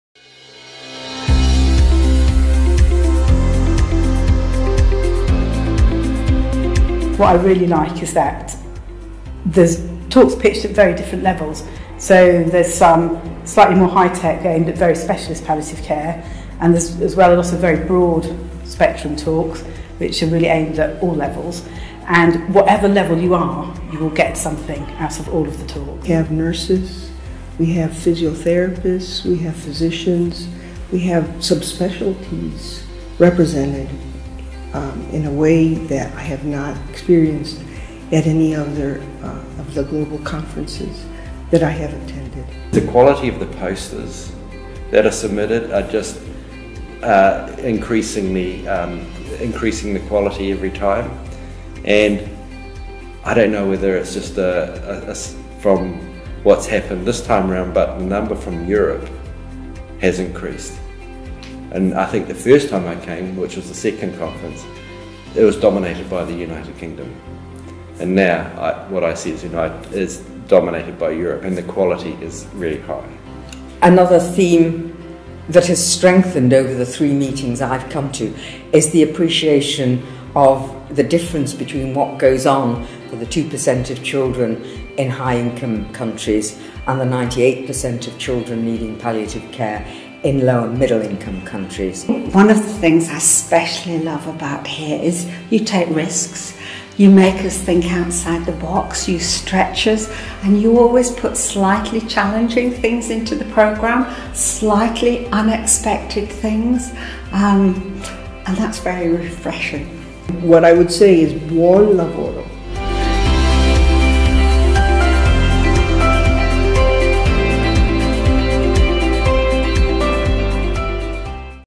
Video Promo 5th Maruzza International Congress_sub eng
5th Maruzza International Congress on Paediatric Palliative Care, Rome 25th-28th May 2022